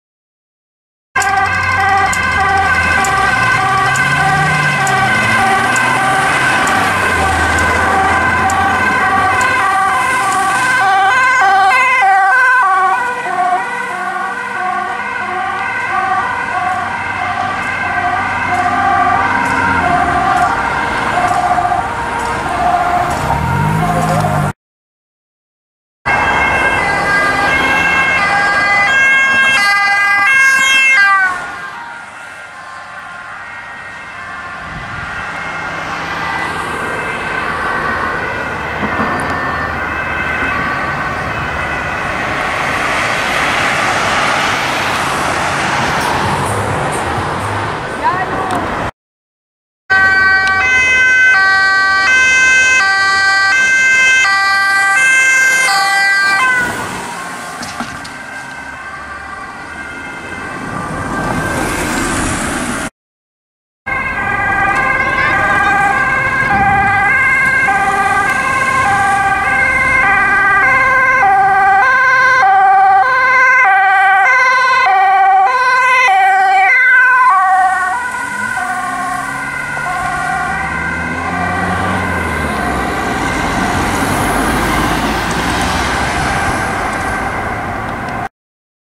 #38 - 4x A1 Ambulances met Spoed in Purmerend
11/07/2017, te Purmerend

Hier ziet u: #38 - 4x A1 Ambulances met Spoed in Purmerend!

Voertuigen die u ziet:
Video 1: A1 Ambulance 13-188 naar de Overlanderstraat
Video 2: A1 Ambulance 13-197 naar de Persijnlaan
Video 3: A1 Ambulance 13-120 naar het Pallas
Video 4: A1 Ambulance 13-198 naar het Abel Tasmanplein

Volg me op Instagram:
cz_emergency

*MW 112Media

PS: De beelden mogen niet zonder toestemming van de eigenaar worden gebruikt!